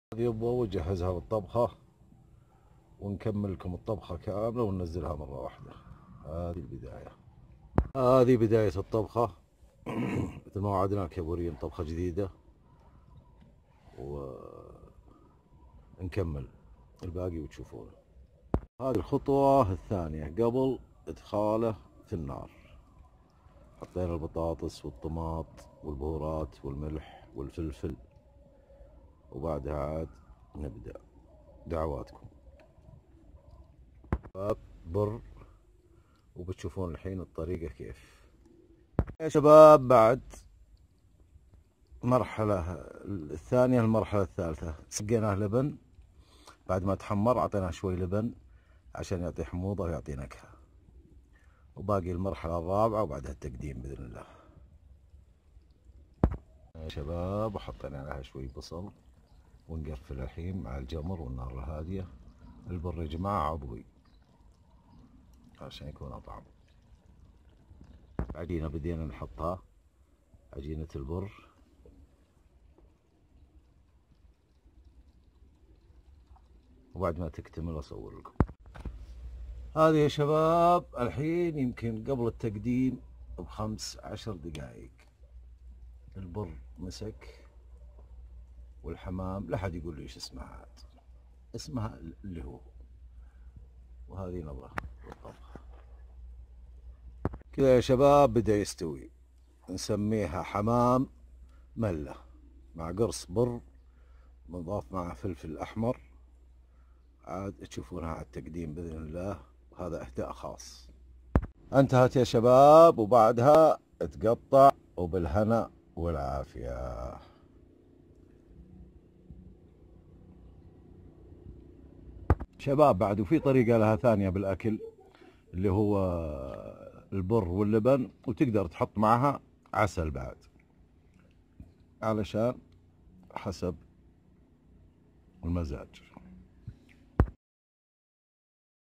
من أغرب طبخات الحمام الزغاليل ـ مع الشيف أبو بـــــدر !!
ملّة الحمام ـ أو اختر لها اسم من عندك .. الأكيد أنها أغرب طبخة حمام بتمرّ عليك!
من إبداعات المنتج الشيف أبو بدر ـ
ماشاء الله تبارك الله :)